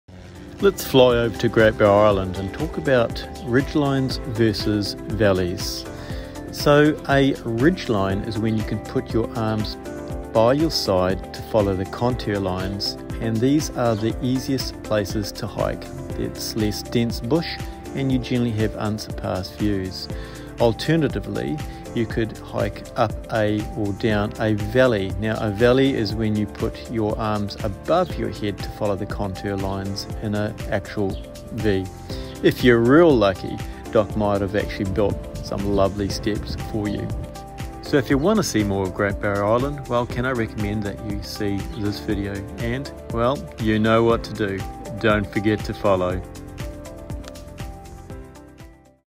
How to read a Map - Ridgelines Vs Valleys
Hey friends,  What is a ridgeline, what is a valley, and which do you take when hiking?

You are in a Valley when...If you put your arms up to follow the terrain on either side of you and it forms a V, you are in a V-alley.  This is where you might find shelter from bad weather, water, and a route out by following the water flow down if you ever get really lost BUT bush can be dense, cold, and dark, and is harder for SAR to see you if you activate a PLB.

You are on a Ridgeline when...If you drop both arms down your sides to follow the terrain on either side of you and your arms form an INVERTED V or ^ then you are on a ridgeline or spur. This is the easiest way to get to a mountain summit or cross over mountainous terrain and the route is often easy to distinguish, animals might also leave a trail to follow, the views are often better and the bush is often less dense.

For more one-minute clips like this go to my playlist Shorts: Hiking in New Zealand